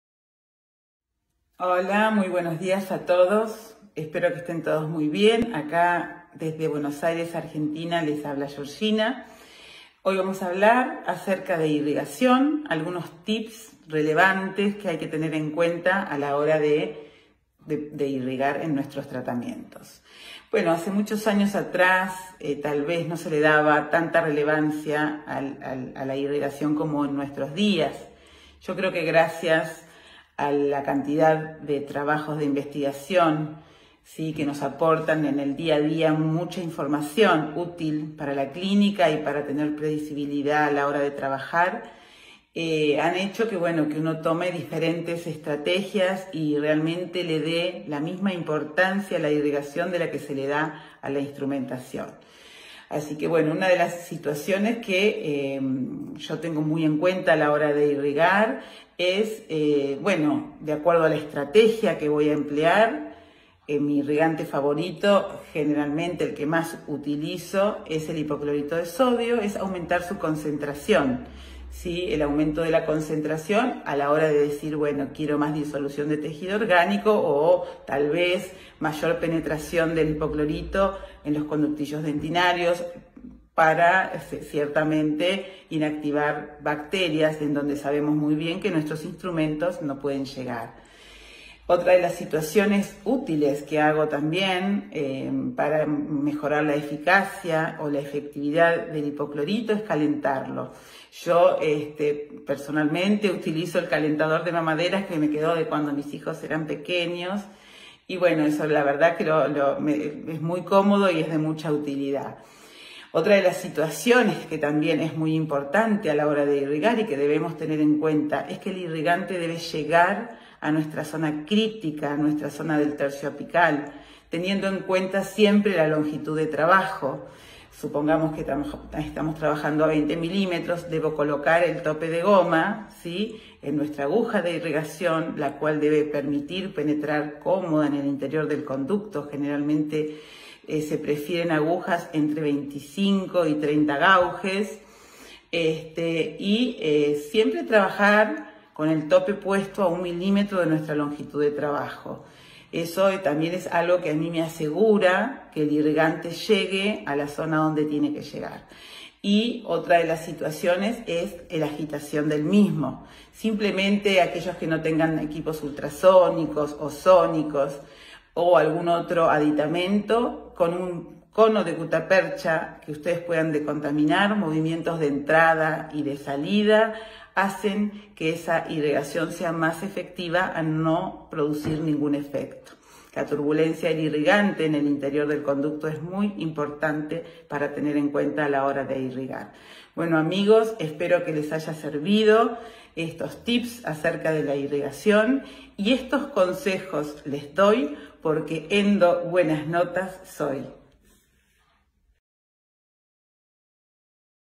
Endo TIP Irrigación
La Dra Georgina Santangelo de Argentina🇦🇷 con nosotros!! Dándonos consejos muy prácticos sobre irrigación.